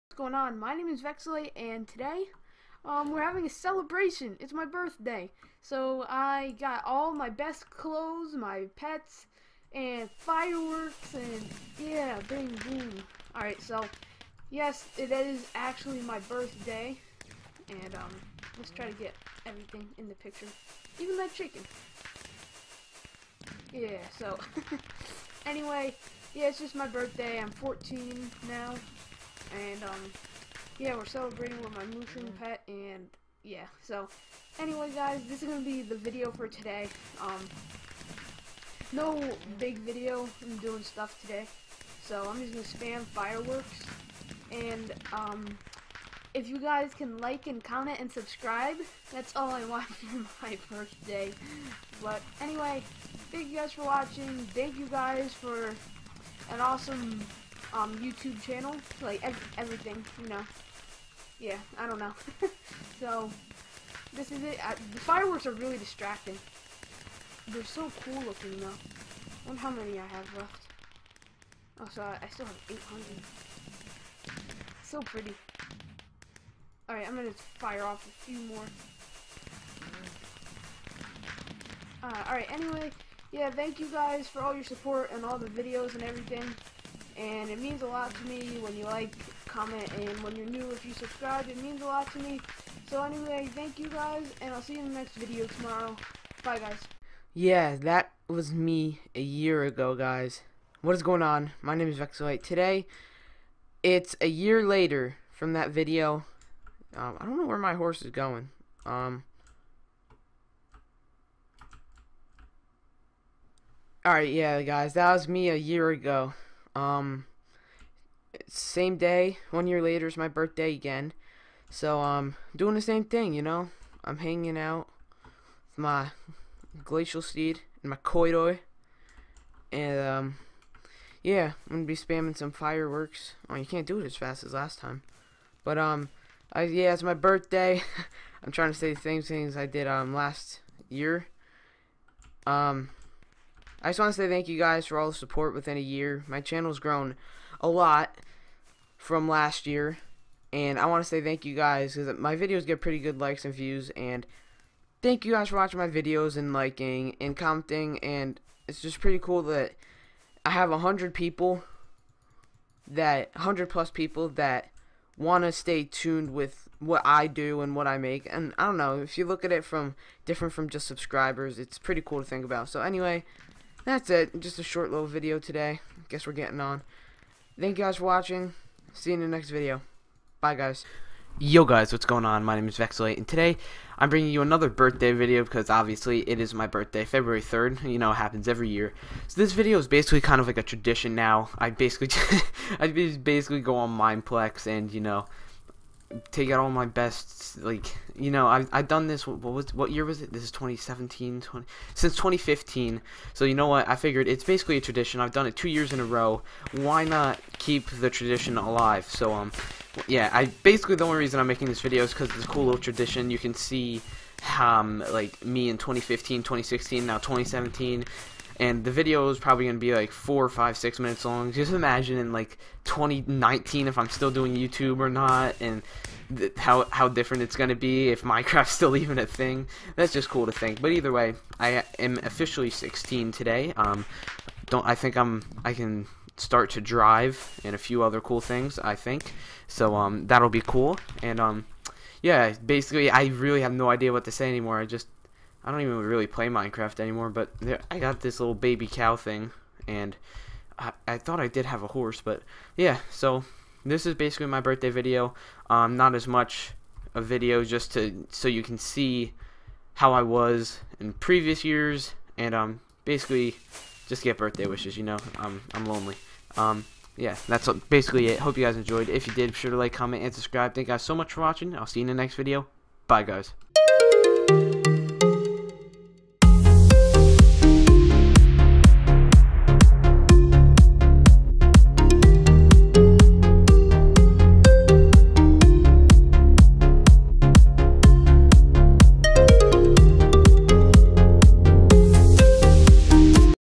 It's My Birthday! (From 2015 to 2016 and 2017!)
In this video I am wishing my self a happy birthday turning 16 on 02/03/17 and comparing it to 2015, 2016 and 2017!

Instagram.com/Vexolyte
Twitter.com/Vexolyte
Twitch.Tv/Vexolyte

Or if you'd like to share me with a friend that would also help a lot!